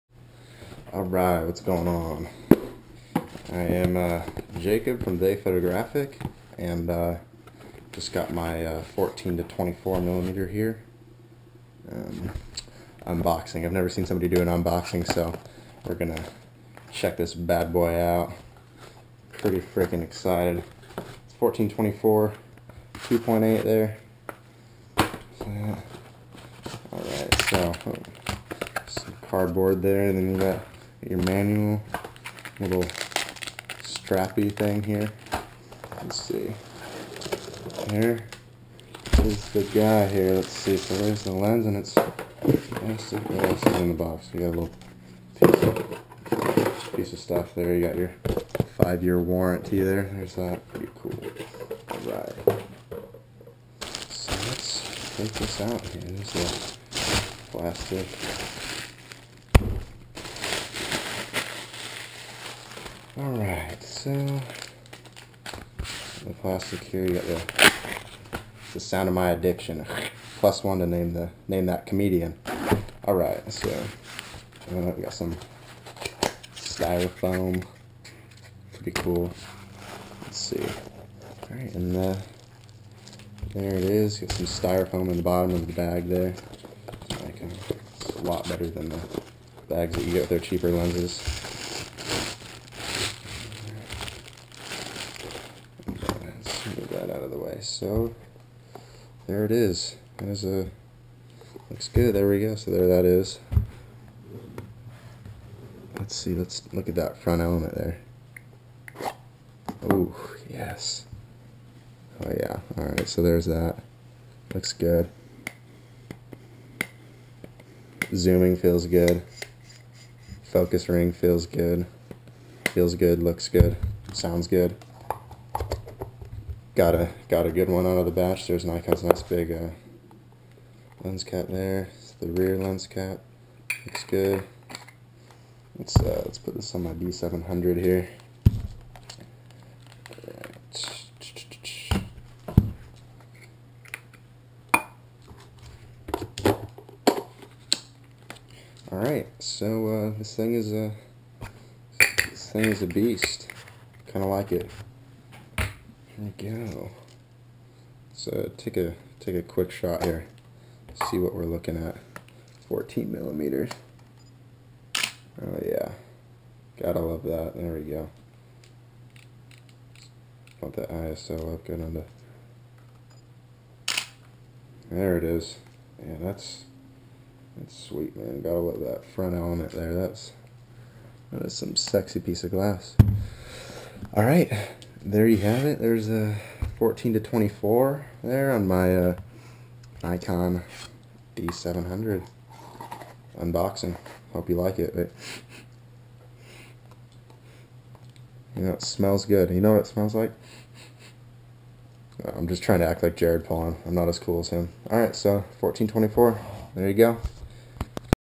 14-24mm F2.8 Unboxing.
alright this is my unboxing of my 14-24mm f2.8. this is my very first professional lens (at least with the nice gold ring around the front :) )